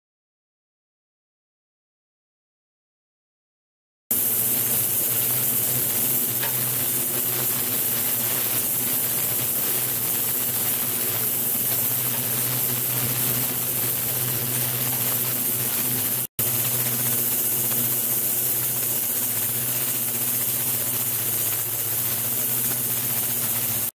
超音波システム（推奨：：超音波システム研究所）
超音波システム（推奨）
１：超音波（28kHz,72kHz)
２：超音波専用水槽(内側寸法　500*310*340mm)
３：脱気・マイクロバブル発生液循環システム
４：超音波の発振タイマー制御

２種類の超音波（振動子）
１：３８ｋHz、７０ｋHz
２：２５ｋHz、３８ｋHz
３：２４ｋHz、６８ｋHz
４：３３ｋHz、２８ｋHz
５：３３ｋHz、４０ｋHz
６：３３ｋHz、７１ｋHz